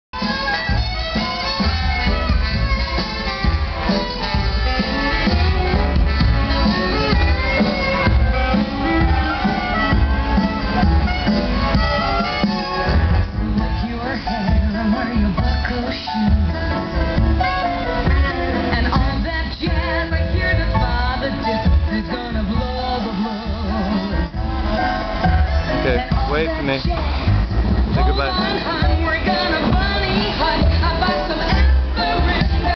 Bellagio Water Show 1
Water show at Bellagio on July 8, 2007 just before midnight.